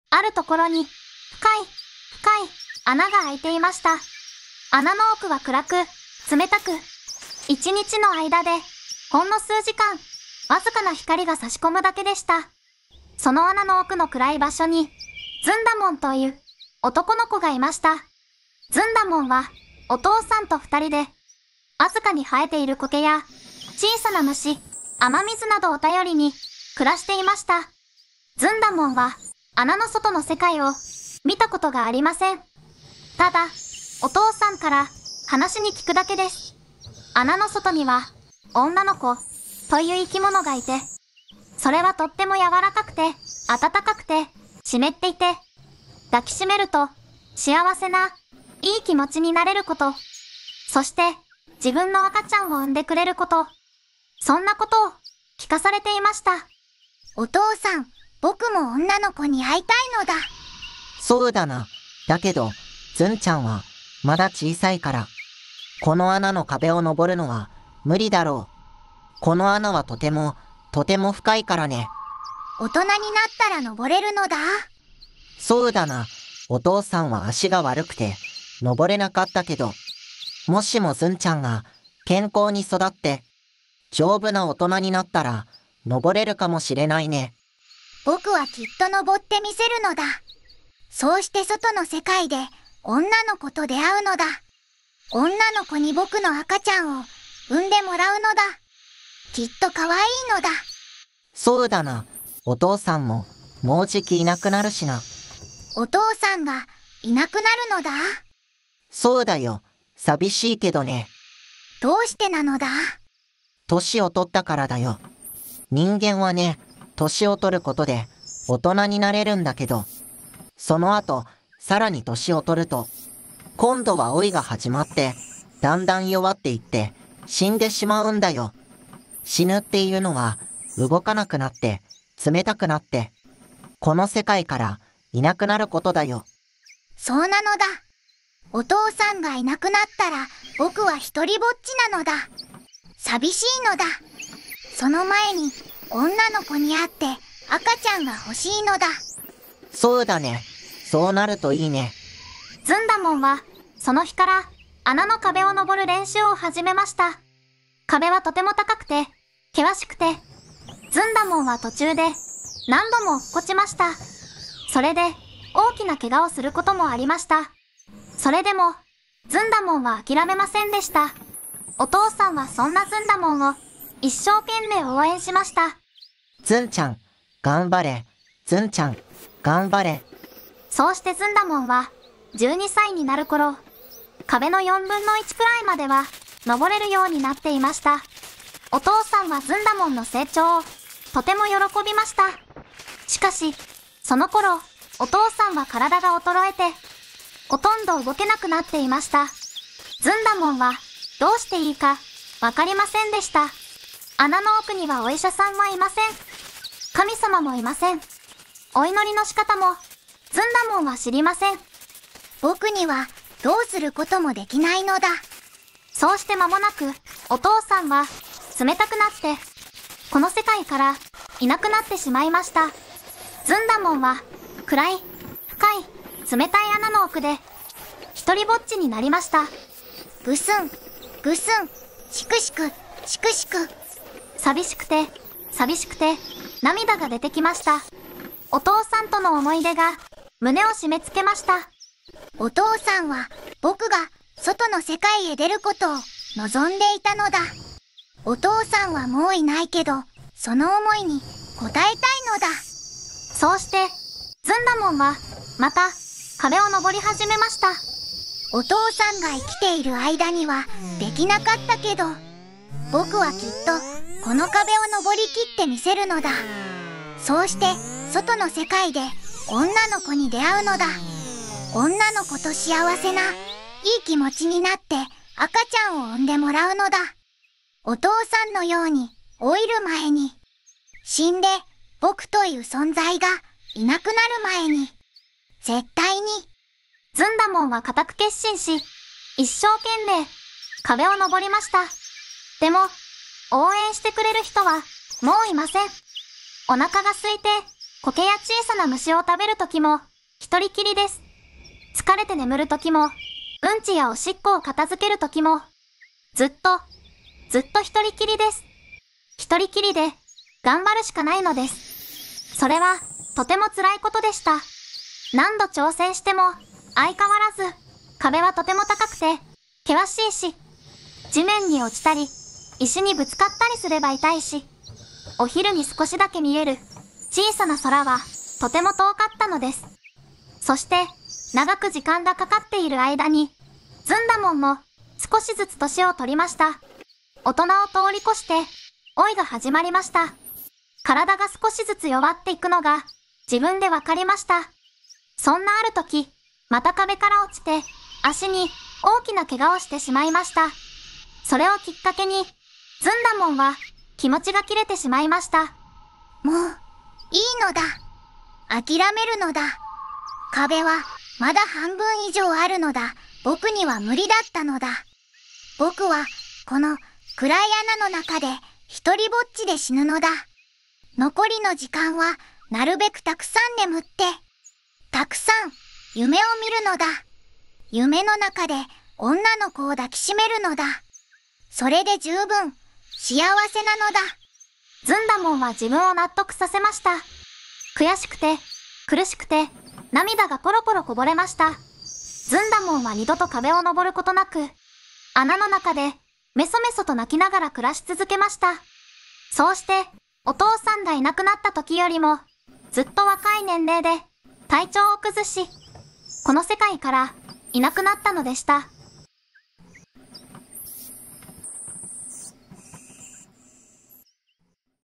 穴暮らしのずんだもん。【ボイボ童話シリーズ】【オーディオドラマ】
ある所に、深い、深い、穴が空いていました。穴の奥は暗く、冷たく、一日の間で、ほんの数時間、僅かな光が差し込むだけでした。

声：
VOICEVOX:ずんだもん
VOICEVOX:白上虎太郎
VOICEVOX:春日部つむぎ

素材：
ずんだもん立ち絵素材改 / 坂本アヒル さんのイラスト - ニコニコ静画 (イラスト)